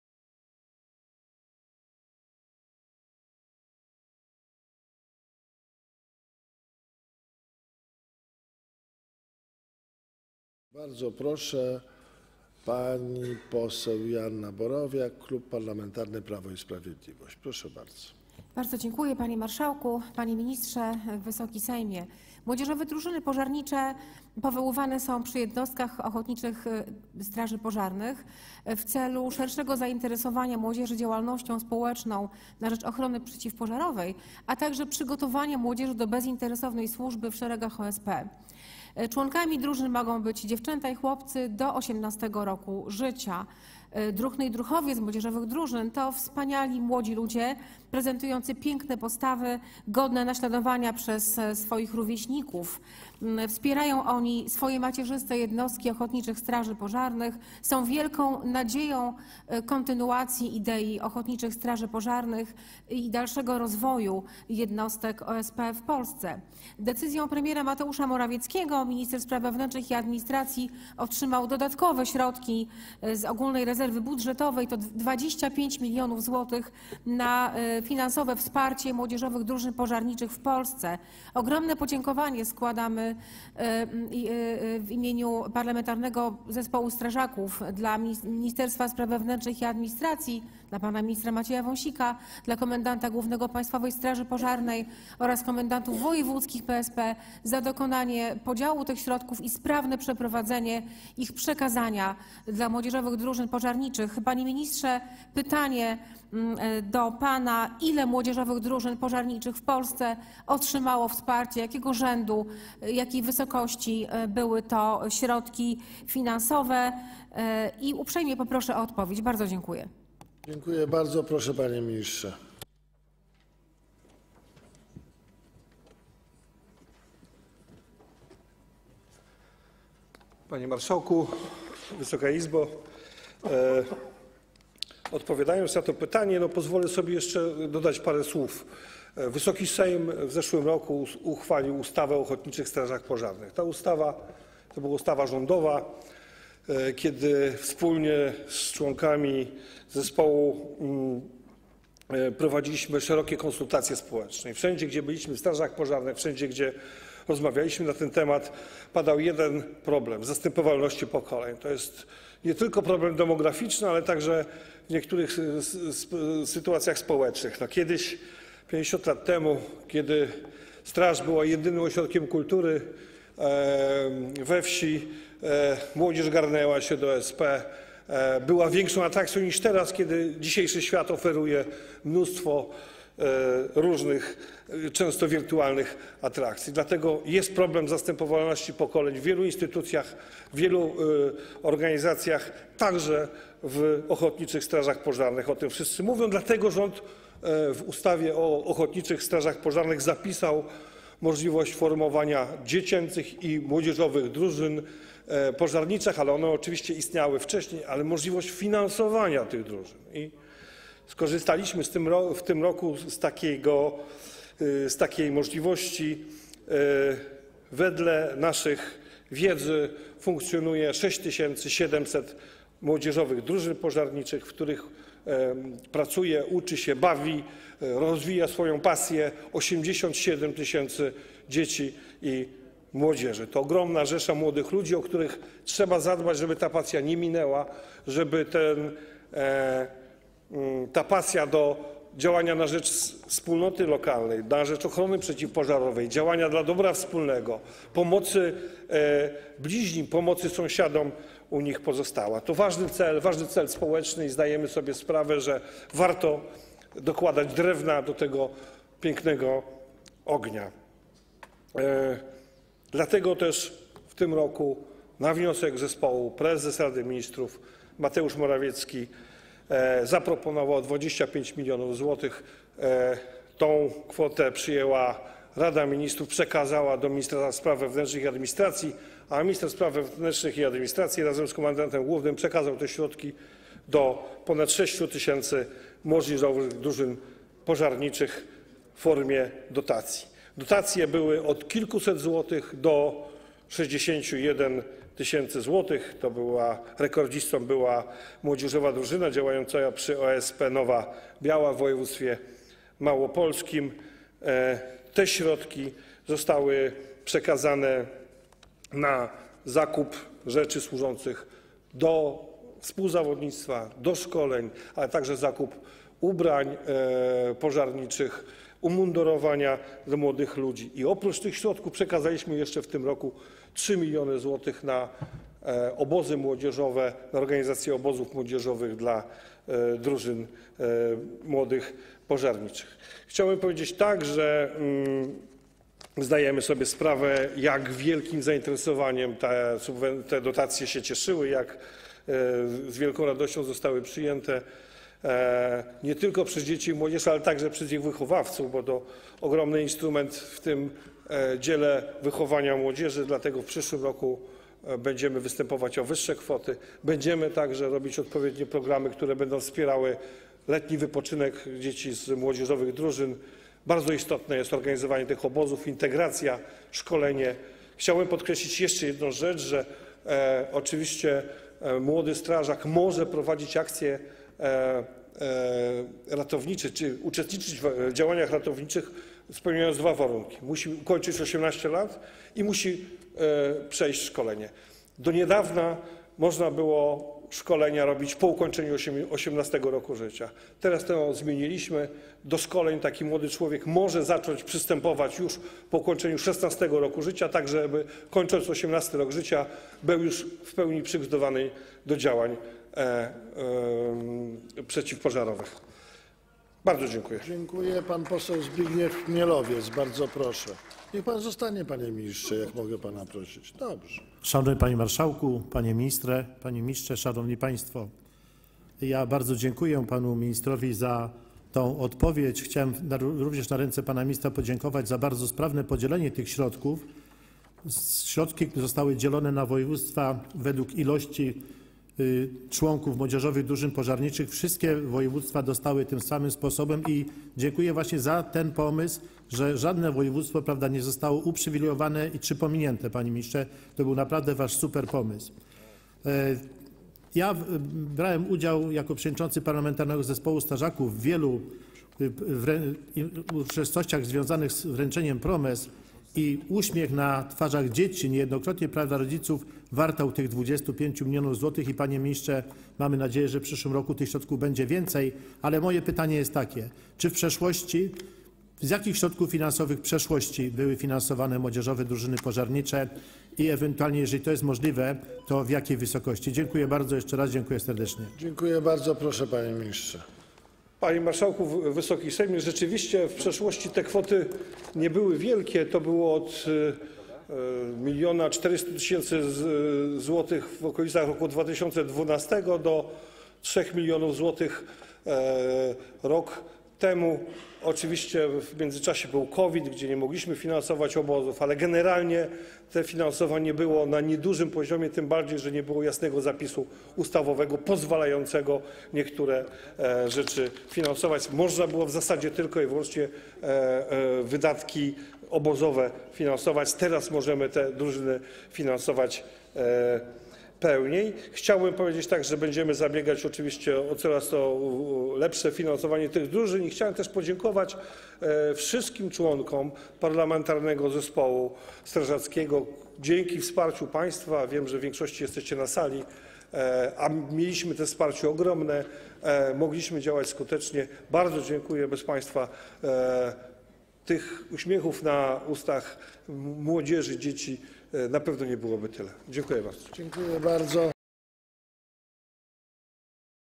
Prawo i Sprawiedliwość - 25 mln zł dla młodzieżowych drużyn pożarniczych
Prawo i Sprawiedliwość - pytanie z 14 grudnia 2022 r.
Pytania w sprawach bieżących

Posłowie Joanna Borowiak, Zbigniew Chmielowiec, Lidia Burzyńska, Anna Milczanowska, Teresa Wargocka, Mariusz Trepka, Anna Kwiecień, Katarzyna Czochara, Iwona Arent, Urszula Rusecka, Przemysław Drabek, Władysław Kurowski, Sławomir Skwarek, Tomasz Zieliński, Maciej Wąsik i Waldemar Andzel - PiS
w sprawie dodatkowego wsparcia finansowego w kwocie 25 mln zł ze środków budżetu państwa dla młodzieżowych drużyn pożarniczych w Polsce
- do Ministra Spraw Wewnętrznych i Administracji.